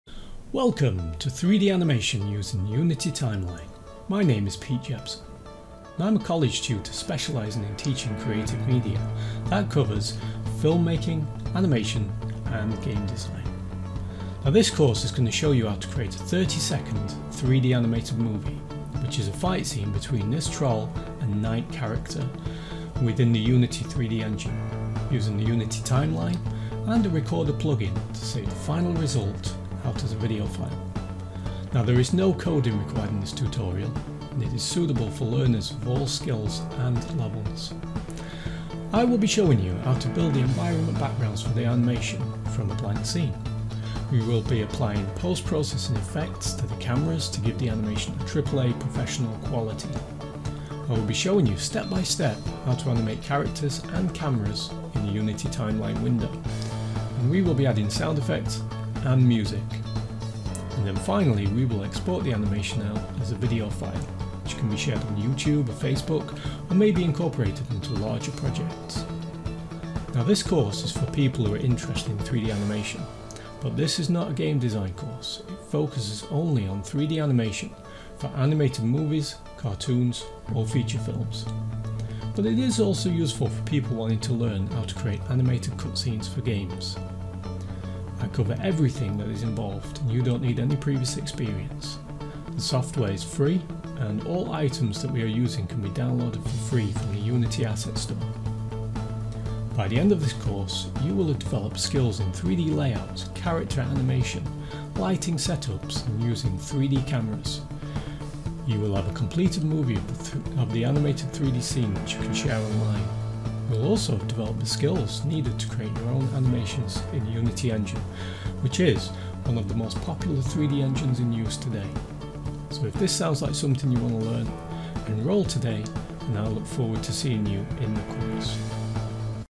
3D Animation using Unity Timeline
A comprehensive course on how to animate AAA quality movies using Unity Timeline.

So, if you want to be an animator, enroll on Udemy today and I look forward to seeing you in the course